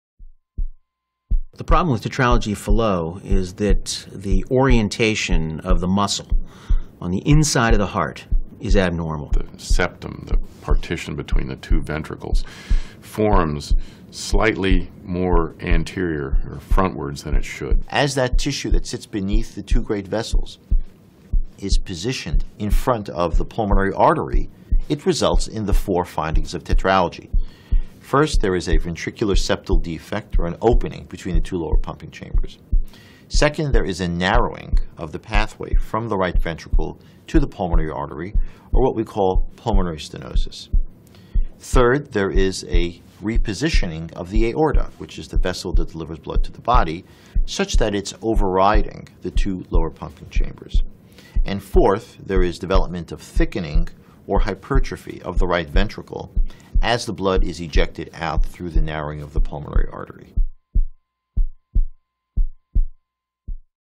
Tetralogia de Fallot
Vídeo explicativo sobre os defeitos existentes na Tetralogia de Fallot.